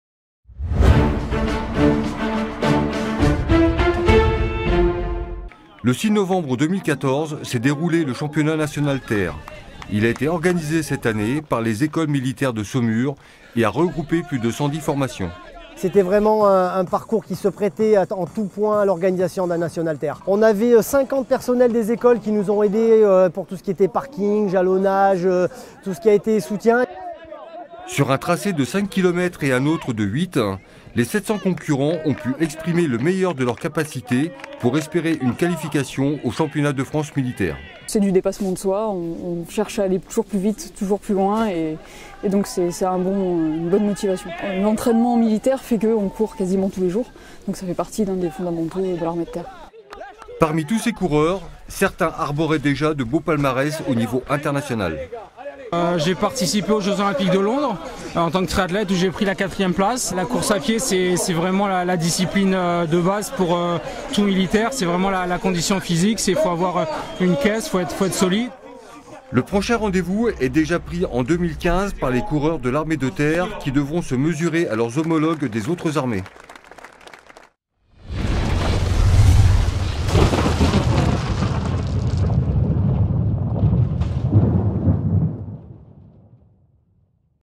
Championnat national Terre 2014 de cross-country
Le championnat national Terre 2014 de cross-country, organisé par les Ecoles Militaires de Saumur, s'est déroulé le 6 novembre.